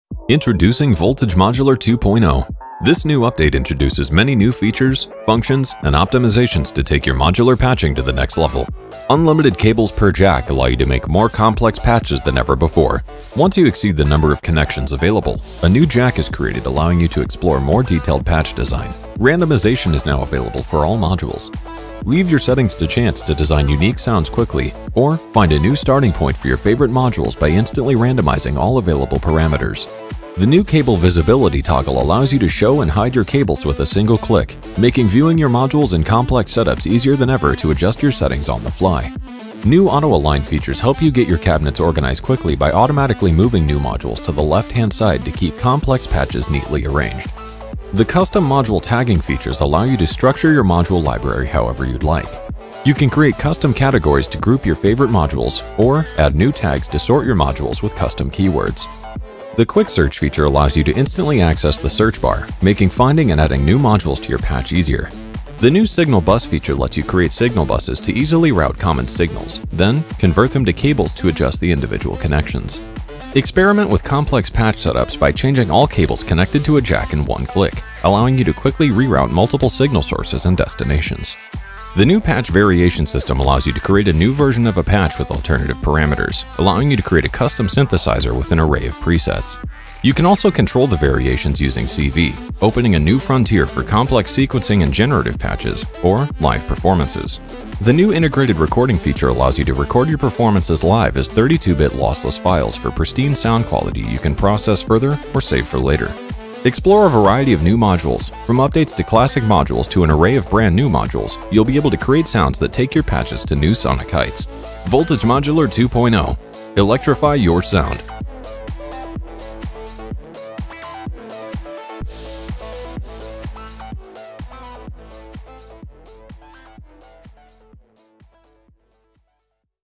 Voltage Modular 2 | Updates & Overview
➥ Learn More About Voltage Modular or Get Your Copy

Voltage Modular 2 is here! This update includes many new features, functions, workflow enhancements, and patching capabilities that allow you to take your patching to new heights.

Dive in with unlimited cables per jack, a suite of new modules and updated modules, explore new patching options, and even record your performances right from your cabinet.

Voltage Modular is a deeply powerful virtual modular platform, designed from the outset to be the best sounding, most flexible, and easiest-to-use virtual modular instrument available.